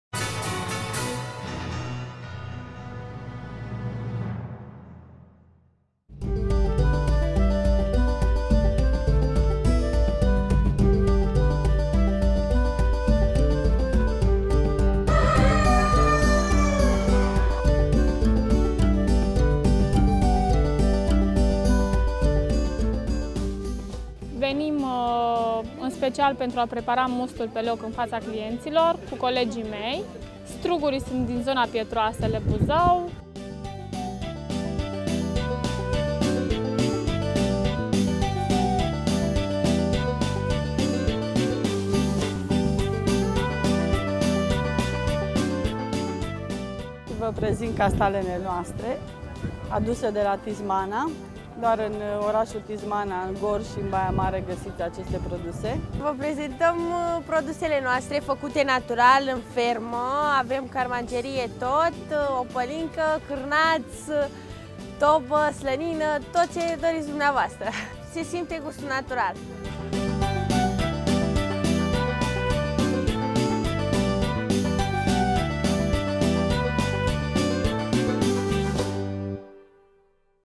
Producători autohtoni la INDAGRA
Sute de producători români au venit la București, la cel mai mare târg de produse şi echipamente în domeniul agriculturii. La INDAGRA sunt expuse utilaje agricole, iar vizitatorii pot admira animalele fermierilor și pot degusta produse tradiționale românești. Cea de-a 21-a ediție a târgului, care are loc la Romexpo, reunește 520 de companii din întreaga lume. INDAGRA se va încheia duminică, iar până atunci sunt așteptați să vină peste 65.000 de vizitatori.